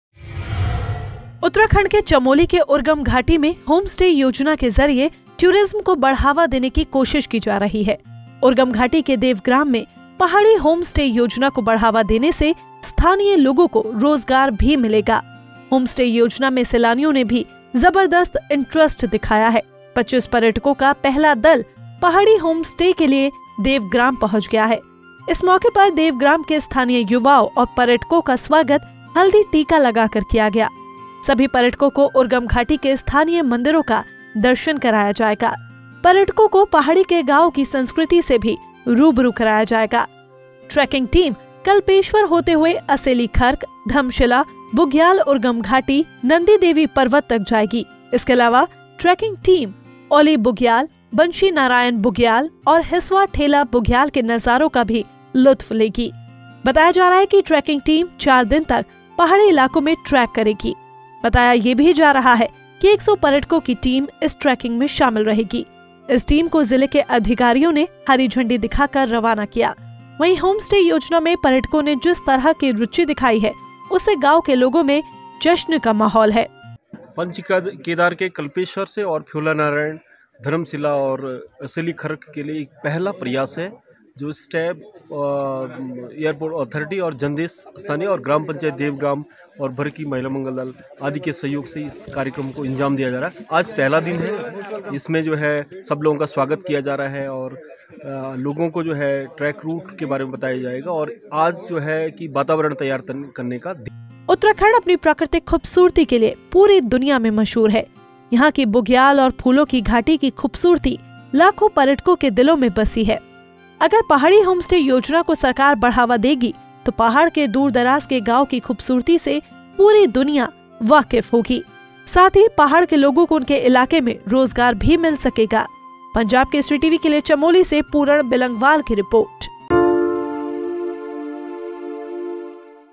CHAMOLI:Home Stay में Interest दिखा रहे हैं Tourist
उत्तराखंड के चमोली के उर्गम घाटी में होम स्टे योजना के जरिए टूरिज्म को बढ़ावा देने की कोशिश की जा रही है. उर्गम घाटी के देवग्राम में पहाड़ी होम स्टे योजना को बढ़ावा देने से स्थानीय लोगों को रोजगार भी मिलेगा. होम स्टे योजना में सैलानियों ने भी जबरदस्त इंट्रेस्ट दिखाया है 25 पर्यटकों का पहला दल पहाड़ी होम स्टे के लिए देवग्राम पहुंच गया है. इस मौके पर देवग्राम के स्थानीय युवाओं और पर्यटकों का स्वागत हल्दी टीका लगा कर किया. सभी पर्यटकों को उर्गम घाटी के स्थानीय मंदिरों का दर्शन कराया जाएगा. पर्यटकों को पहाड़ी के गांवों की संस्कृति से भी रूबरू कराया जाएगा. ट्रैकिंग टीम कल्पेश्वर होते हुए असेली खर्क, धमशीला बुग्याल,उर्गम घाटी, नंदा देवी पर्वत तक जाएगी. इसके अलावा ट्रैकिंग टीम औली बुग्याल, बंशी नारायन बुग्याल और हिस्वा ठेला बुग्याल के नजारों का भी लुत्फ लेगी. बताया जा रहा है कि ट्रैकिंग टीम चार दिन तक पहाड़ी इलाकों में ट्रैक करेगी. बताया जा रहा है कि एक सौ पर्यटकों की टीम इस ट्रैकिंग में शामिल रहेगी.